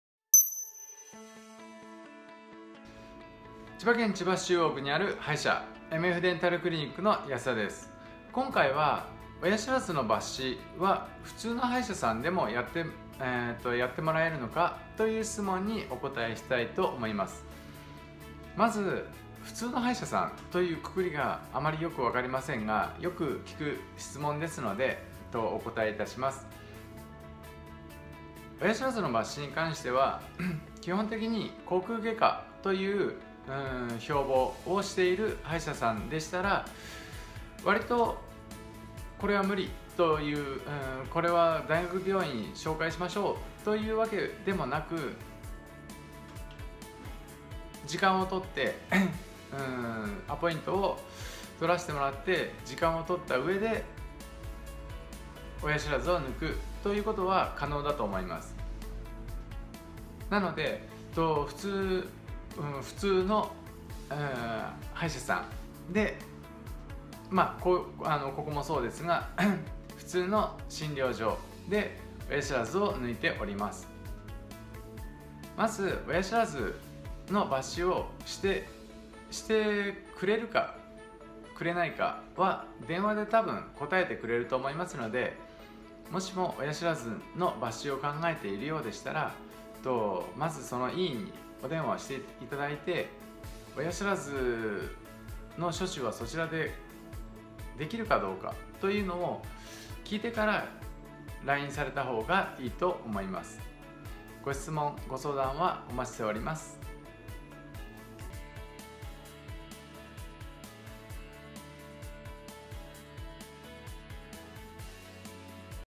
親知らずの手術は、普通の歯医者さんでもやってくれるの？【千葉市中央区の歯医者】
〒260-0021
◆電話◆
◆診療科目◆
◆診療日時◆
平日　9:00~13:00、15:00~21:00
土曜　9:00~13:00